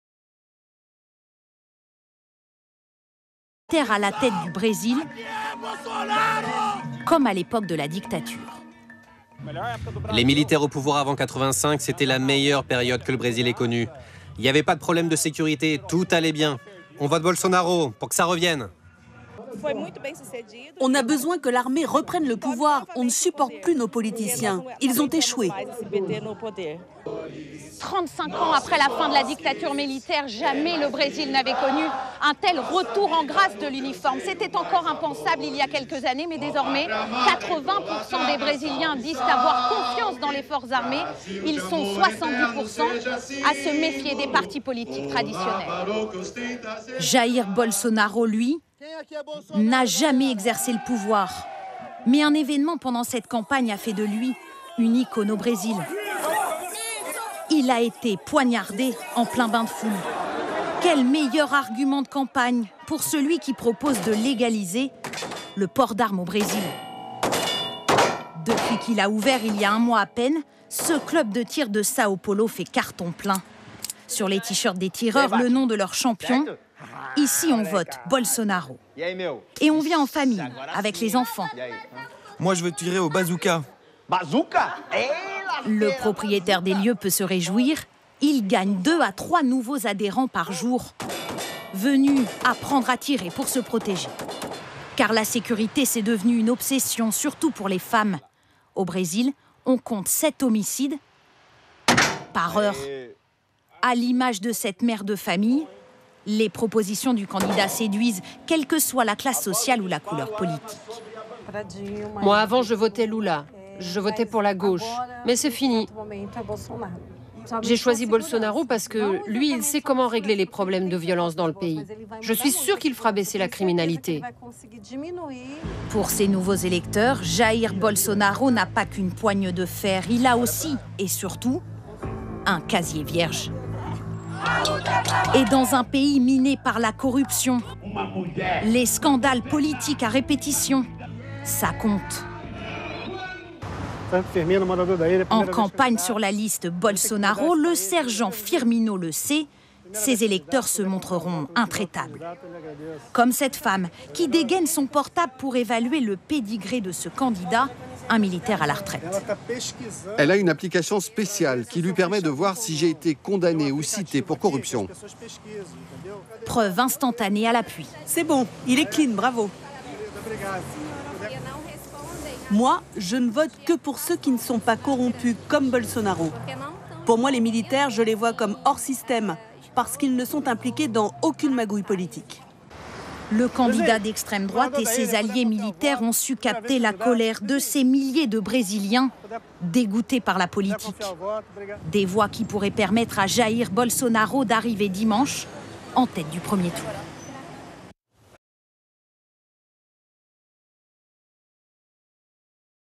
Brésil : le retour des militaires
Au Brésil, à 3 jours du 1er tour de la présidentielle, Jair Bolsonaro continue de progresser dans les sondages. Le candidat de l'extrême droite recueille 32% des intentions de vote. L'ancien militaire promet de réduire la dette et privatiser.